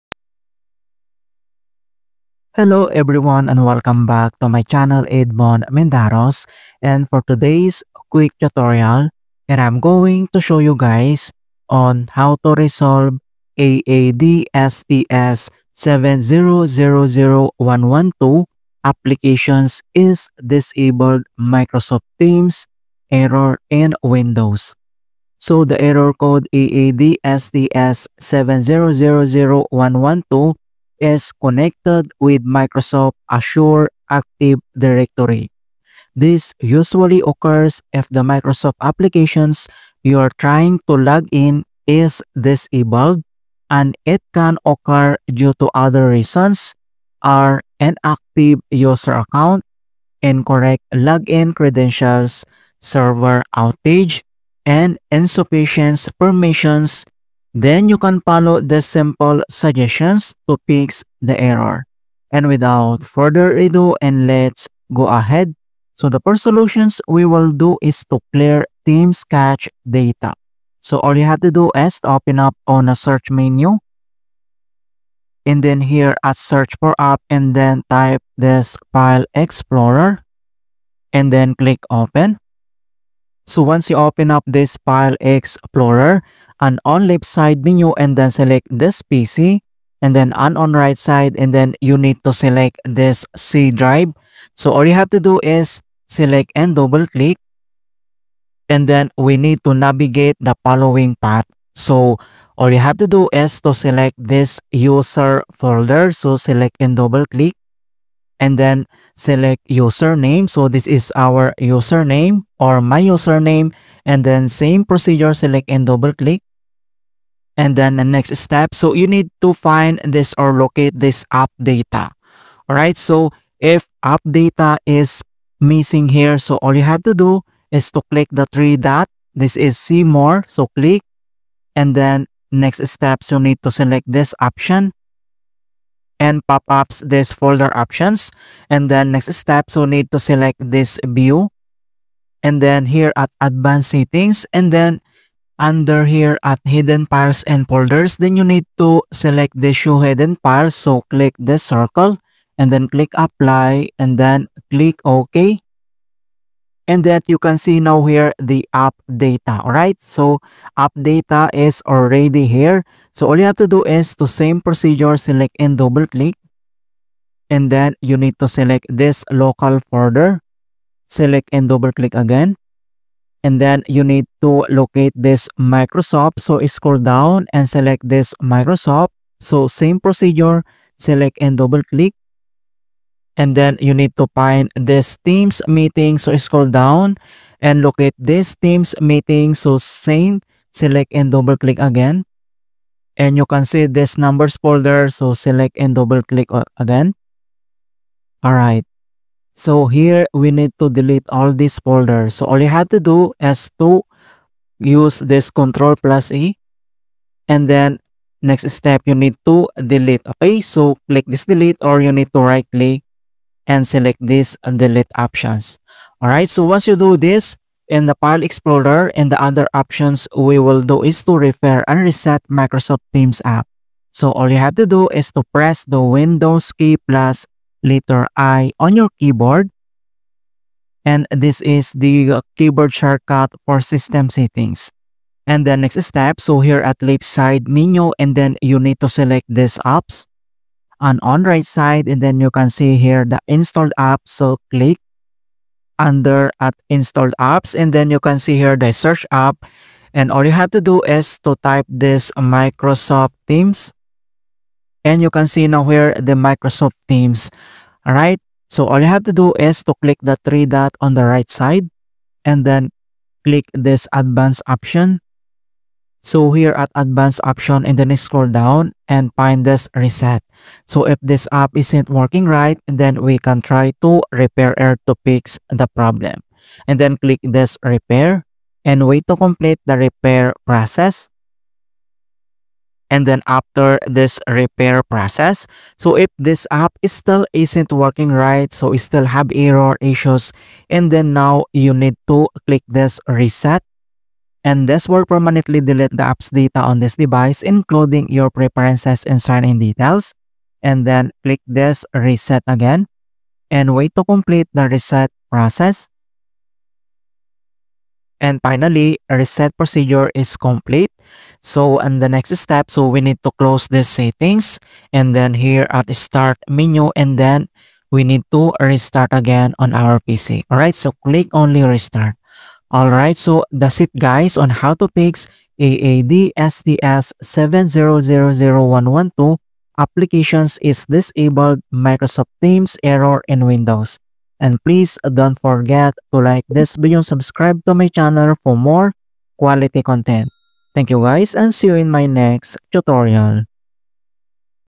How To Fix AADSTS7000112 Application Is Disabled Microsoft Teams Error in Windows
If the AADSTS7000112, Application is disabled Microsoft Account Sign-in error keeps troubling you, then this post may help. This error can disrupt seamless access to Microsoft accounts, causing frustration and decreasing productivity. The complete error message reads: Sign in Sorry, but we’re having trouble signing you in. AADSTS7000112: Application is disabled.

The error code AADSTS7000112 is connected with Microsoft’s Azure Active Directory. This usually occurs if the Microsoft application you’re trying to log in is disabled. However, it can occur due to other reasons. Some of them are: Inactive user account, Incorrect login credentials, Server outages and Insufficient permissions.

Clear Teams Cache Data:
1. Press Windows + R to open the Run dialog box.
2. Enter the following path and hit Enter. users\username\Appdata\Local\Microsoft\Teams
3. Here, press Ctrl + A to select all the files and then press Shift + Del to delete them permanently.
4. Restart your device, try logging in again and see if the error’s fixed.

Repair or Reset Teams App:
1. Click on Start and pick Settings.
2. Go to the Apps section.
3. Locate Microsoft Teams from the list.
4. Click on the three vertical dots and pick Advanced options.
5. Click Terminate to ensure that the app is not running.
6. Under the Reset section, click Repair and see if that solved your issue.
7. If not, then try the Reset option.